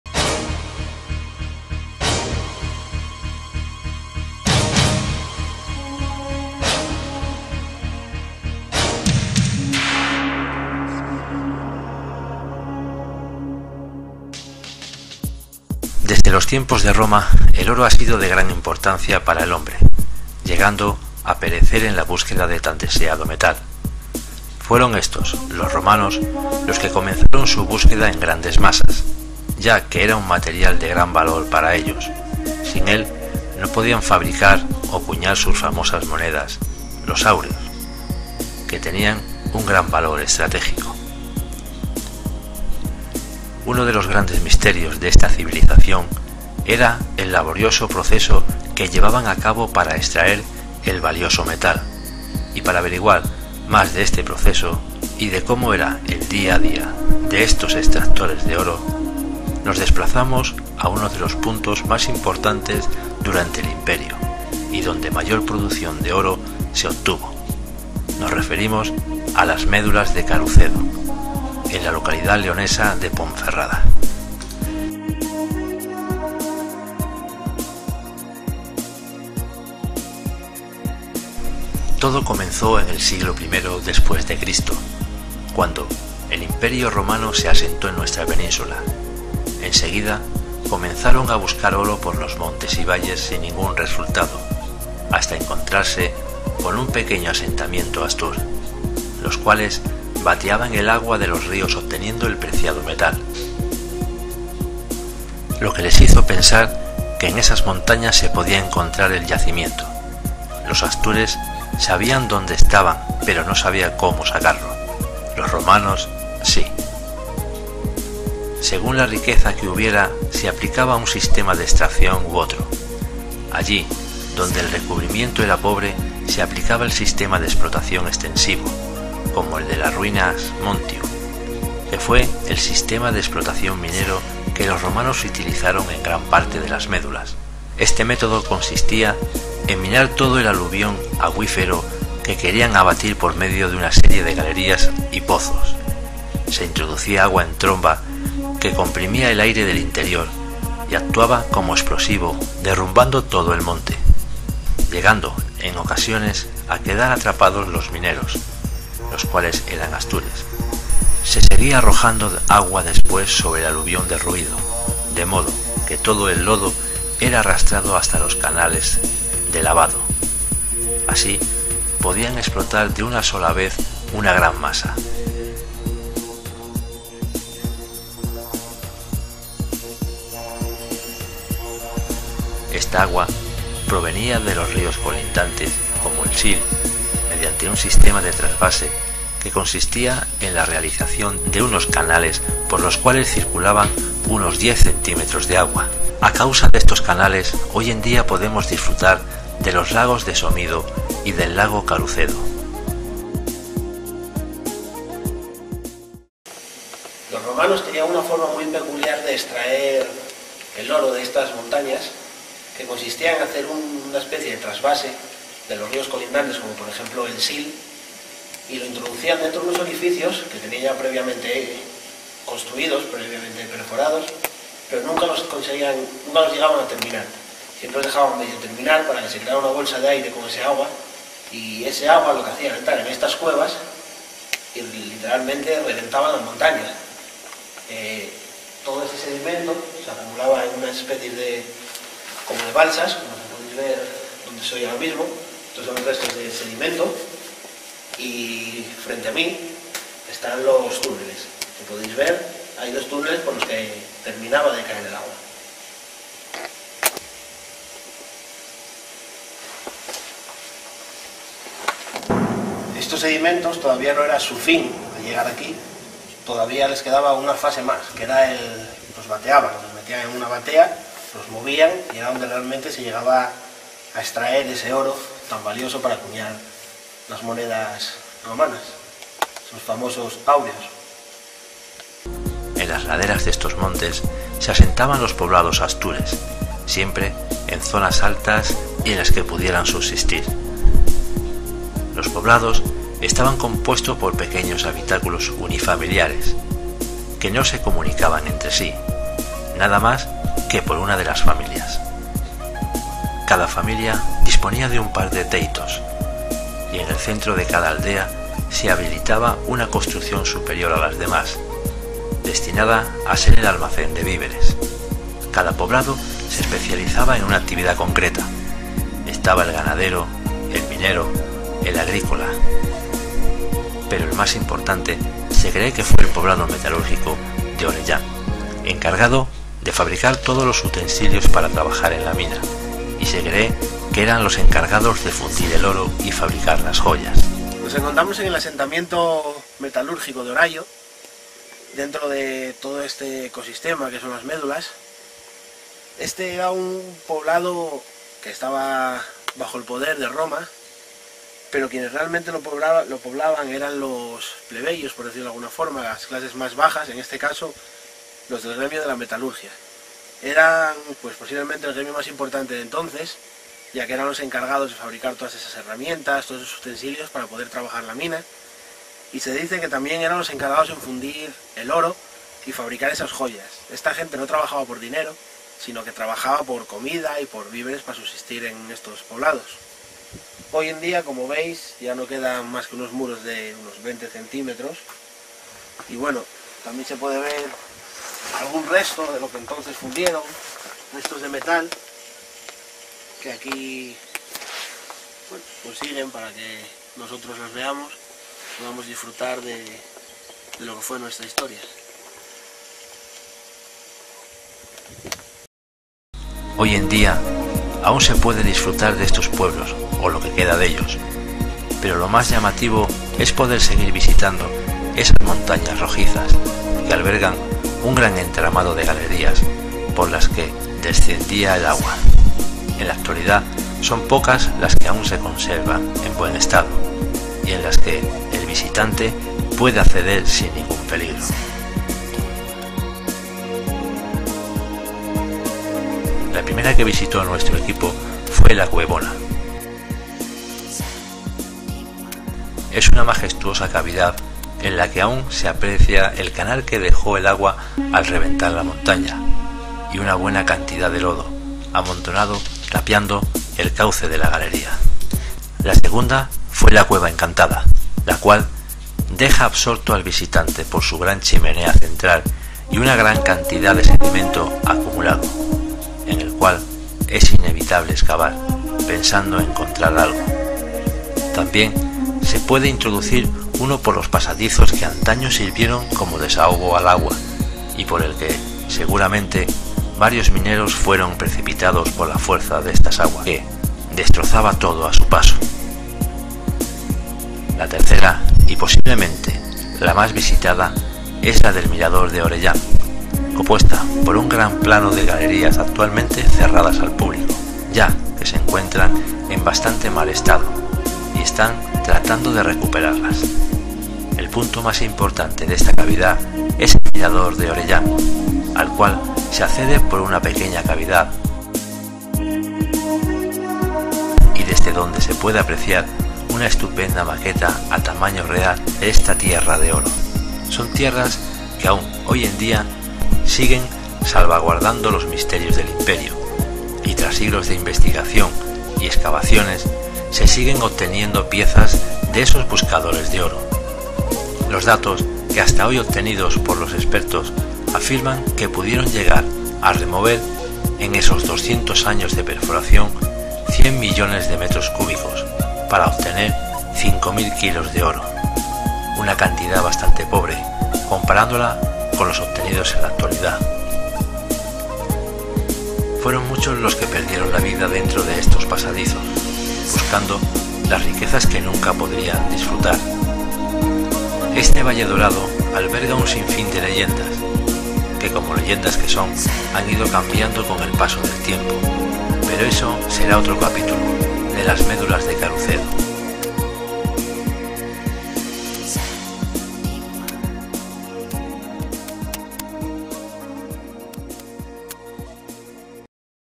LAS MEDULAS
Las Medulas de Carucedo, Ponferrada. Un lugar en que antaño, los romanos buscaban oro en nuestra península y donde hoy día, aun se puede encontrar ese preciado metal. Lugar mágico y lleno de leyendas.
PD. Dejanos tu comentario para ir mejorando. Un saludo